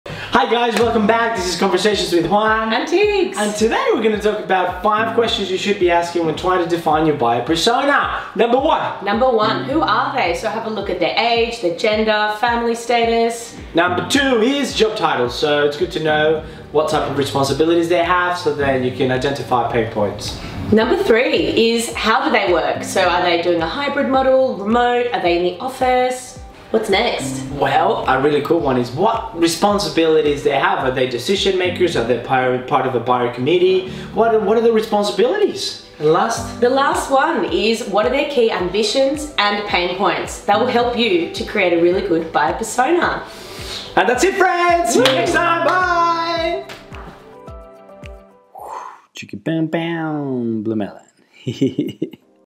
Creating Effective Buyer Personas: 5 Vital Questions for Targeted Marketing | BlueMelon Insights
Unlock the Power of Buyer Personas! In this episode of Conversations with Juan & Tigs, we unveil the 5 crucial questions to supercharge your buyer research and persona development.

Crafting buyer personas is key to understanding your audience deeply and tailoring your strategies. Whether you're familiar with user research, buyer persona creation, or user personas, our insights will fuel your success.

Let's discuss how to align our expertise with your project goals

Enhance your marketing precision and hit your targets like never before.